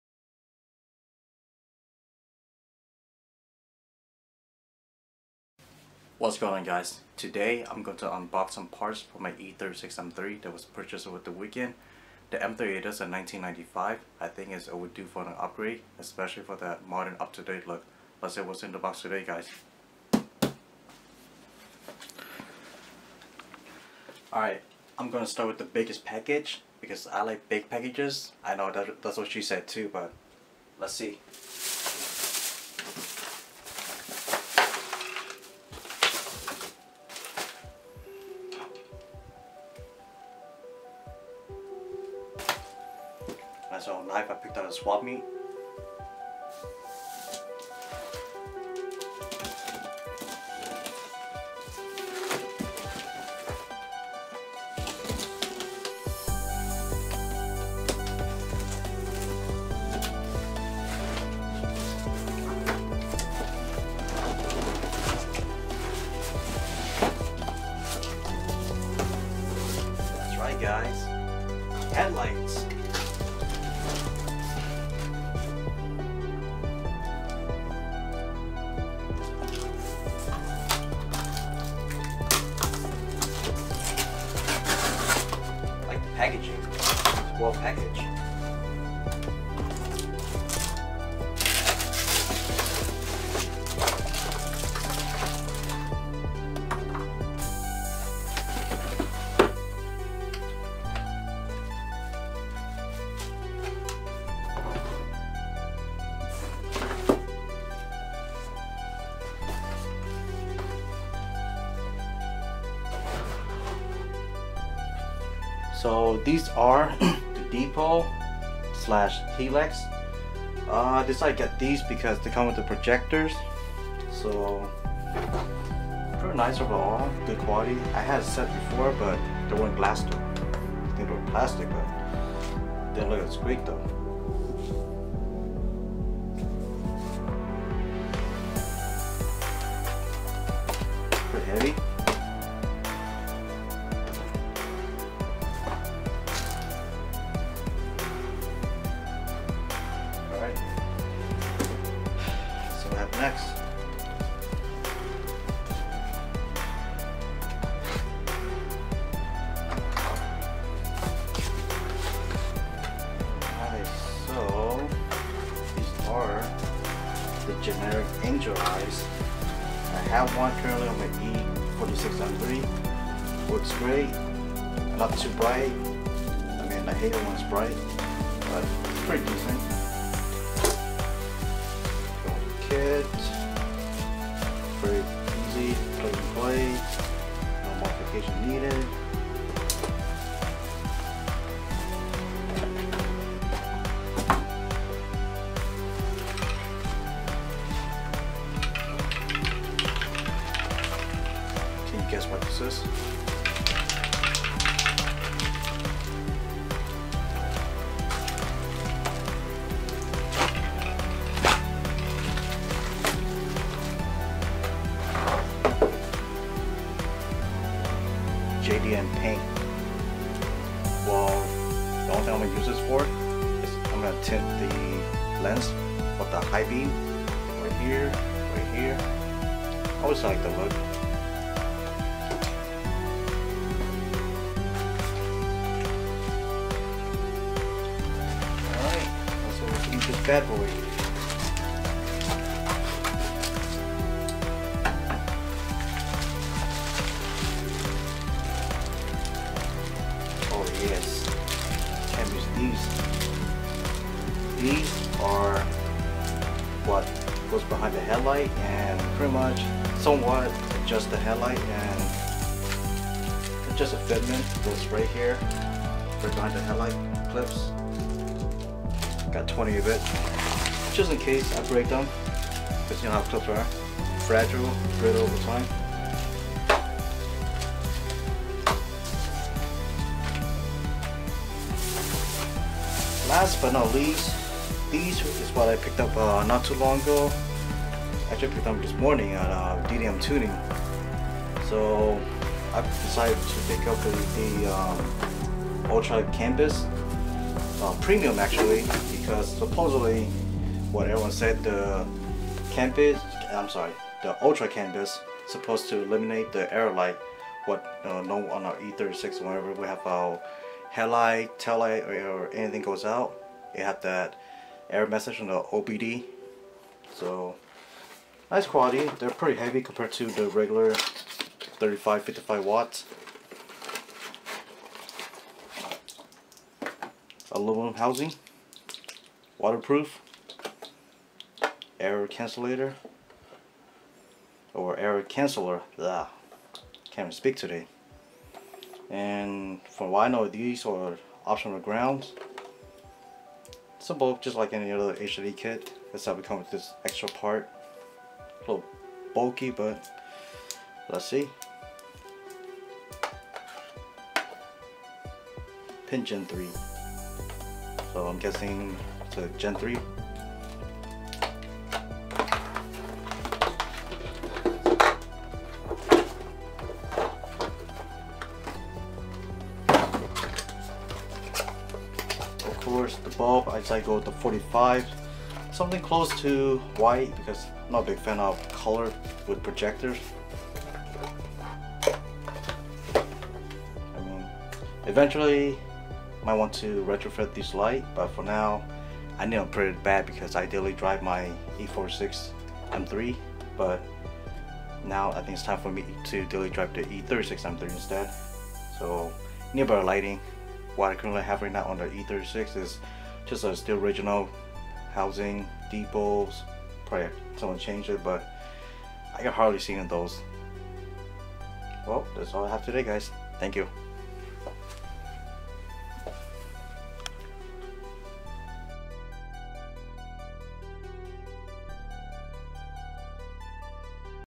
Unboxing parts for my BMW E36 M3
Unboxing parts for my 1995 BMW E36 M3. Check out link below for items that were unboxed.
BMW E36 DEPO Projector Headlights

SMD Angel Eyes

CCFL Angel Eyes

JDM Japan Yellow tint spray

DEPO Smoke Corner Lights for Coupe/Convertible (Best quality for E36)

DEPO Smoke Corner Lights for Sedan (Best quality for E36)

DEPO Clear Corner Lights for Sedan (Best quality for E36)

DEPO Clear Corner Lights for Coupe/Convertible (Best quality for E36)